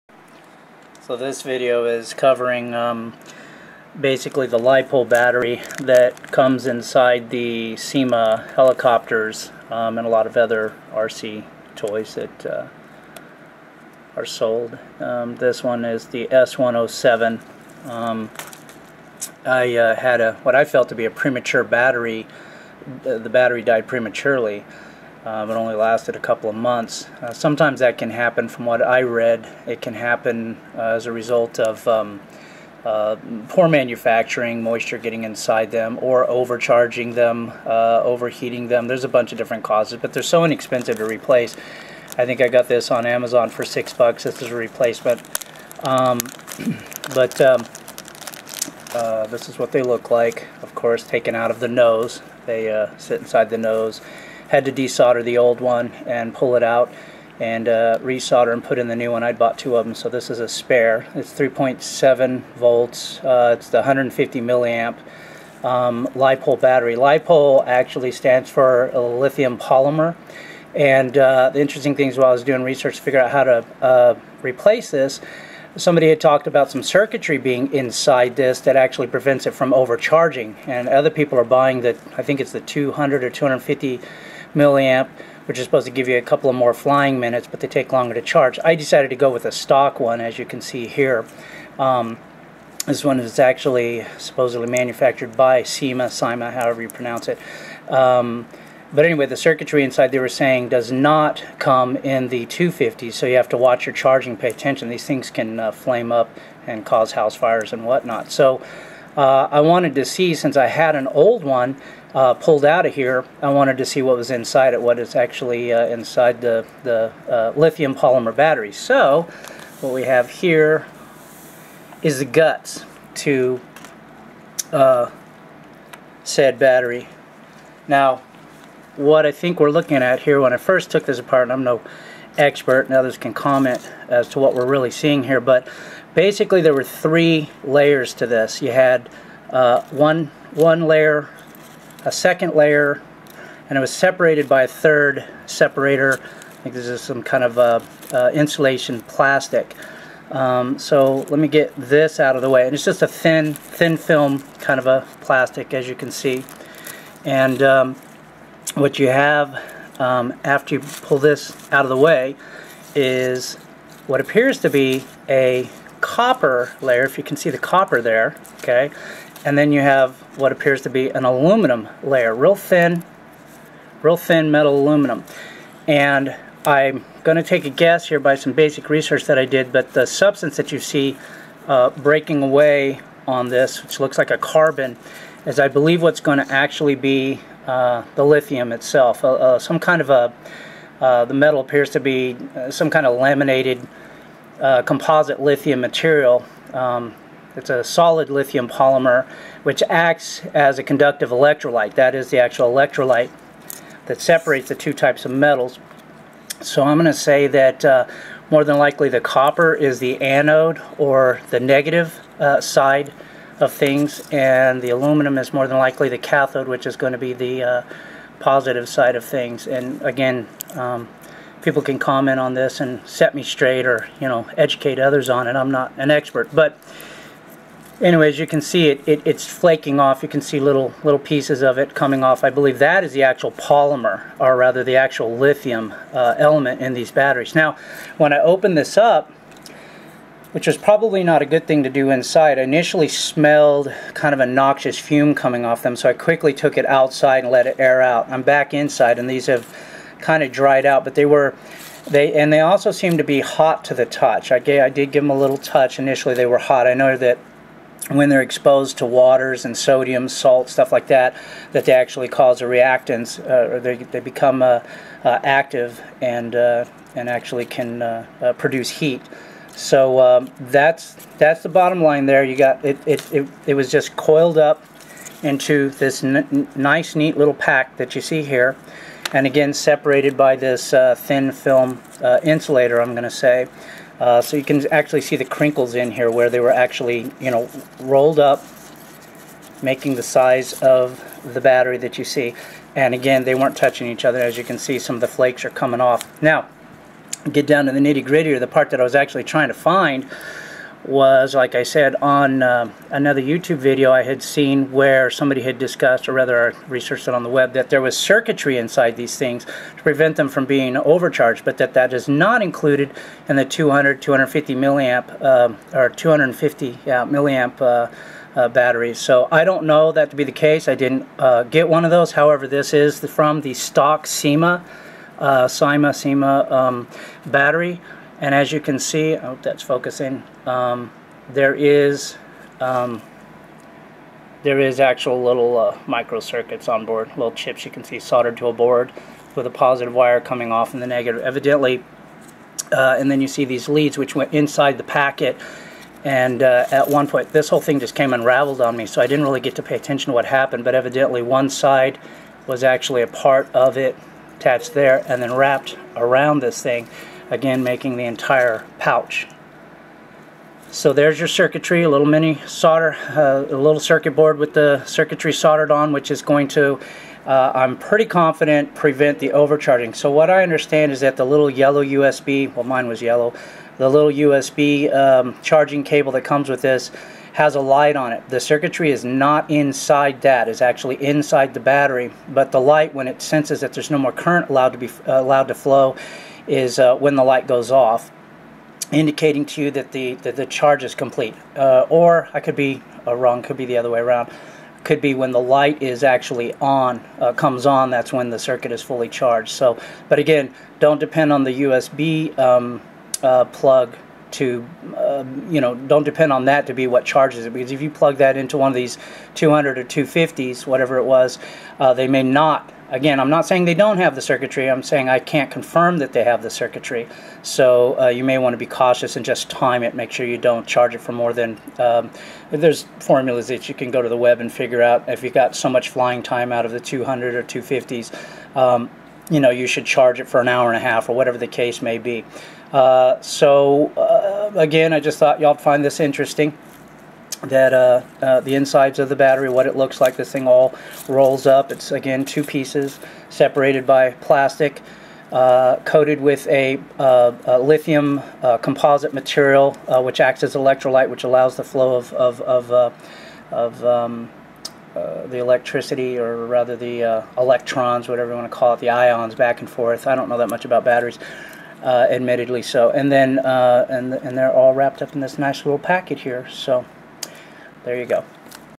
Inside the Syma Helicopter LiPo Battery
See what's inside of a Syma Helicopter battery. This video shows the onboard circuitry and the composition of materials inside your average Li-Po battery used in hobby craft such as the popular Syma S107 Helicopter.